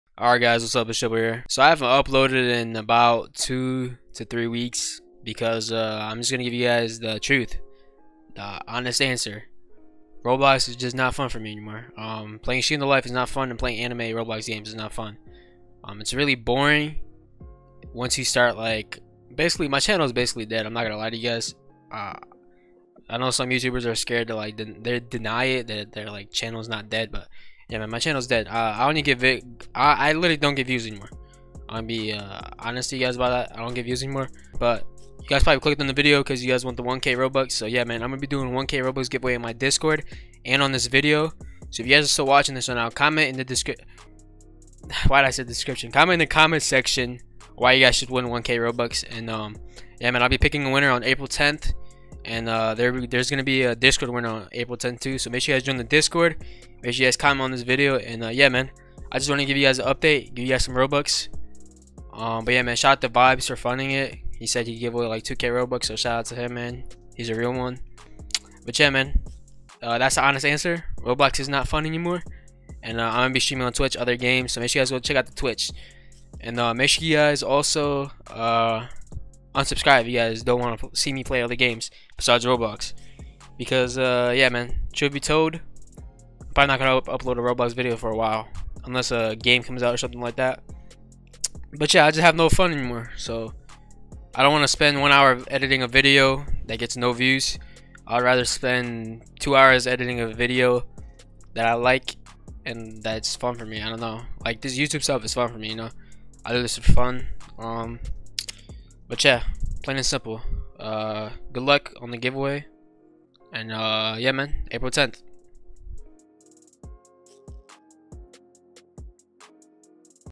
[2000 ROBUX GIVEAWAY] why I don't upload
ENDS ON APRIL 10

1k will be given away on this video enter with the instructions below

2. comment why you should win the robux

example - I should win the robux because i want to buy the private server gamepass in shindo life